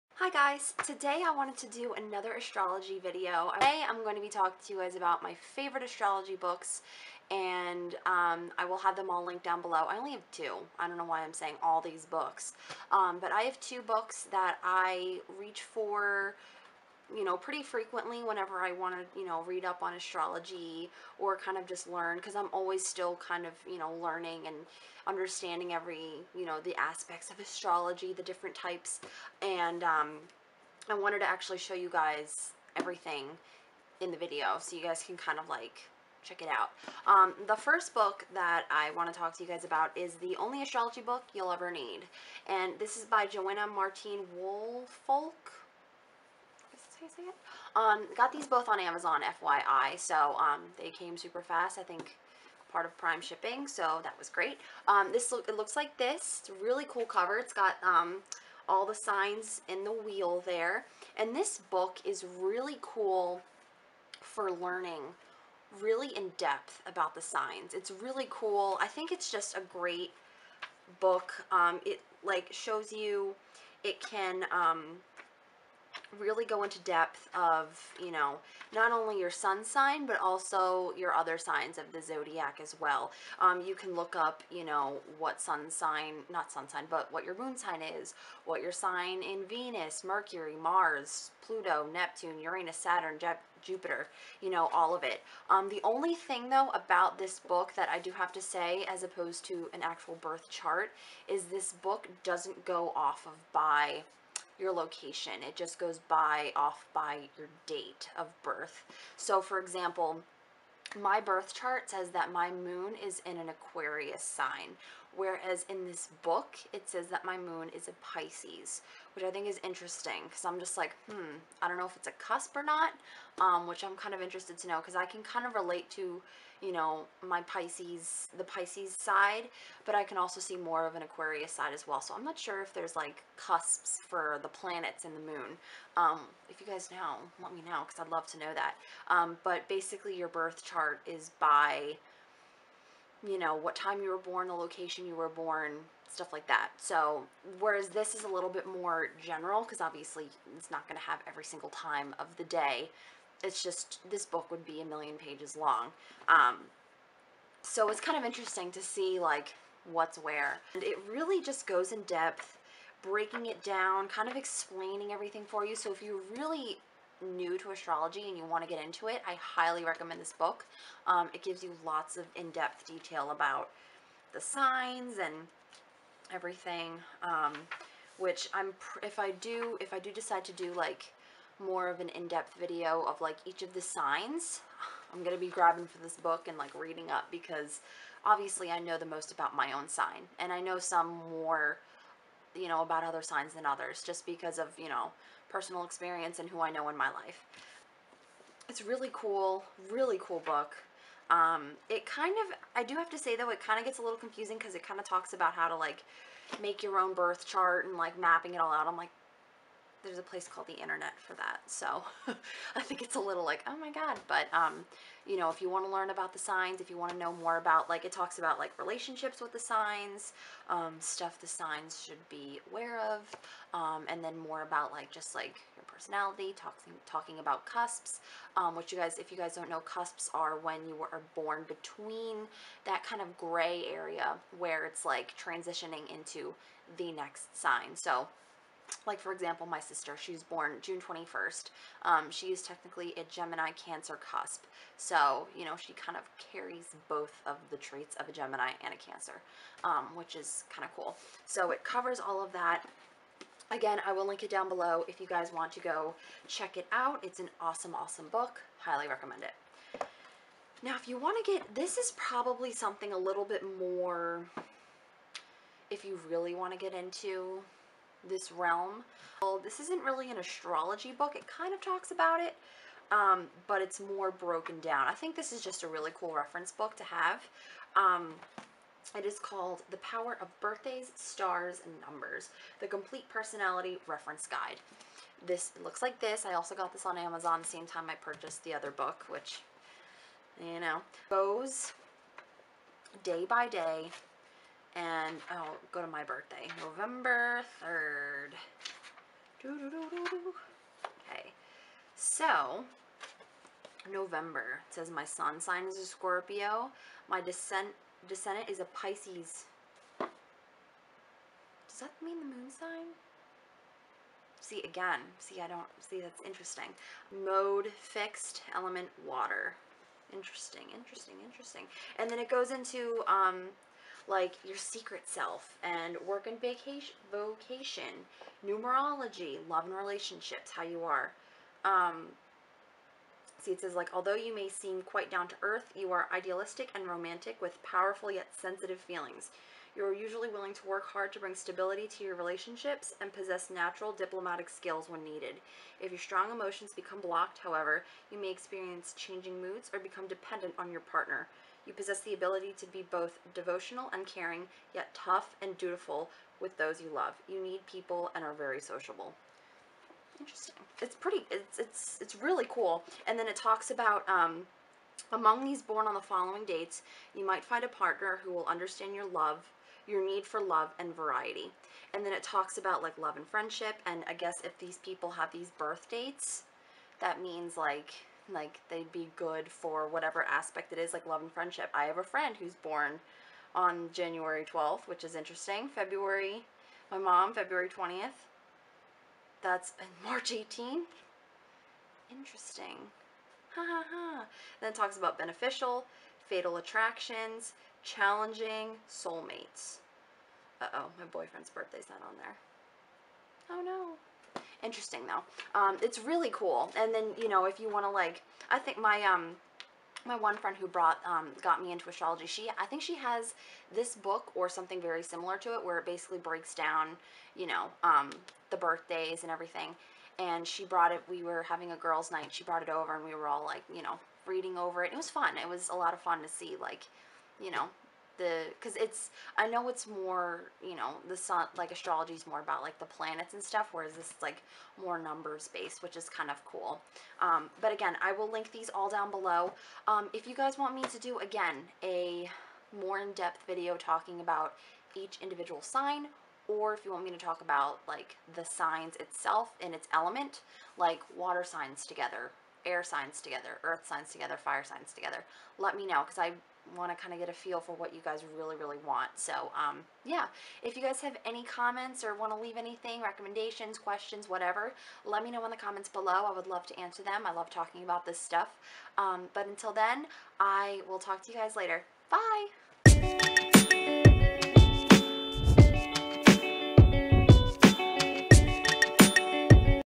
My Favorite Astrology Books!
Hello Again!

Today I wanted to share with you guys my favorite Astrology books!

The Only Astrology Book You'll Ever Need!:

The Power Of Birthdays, Stars, & Numbers!:

The products shown in this video were purchased on my own. All opinions are my own.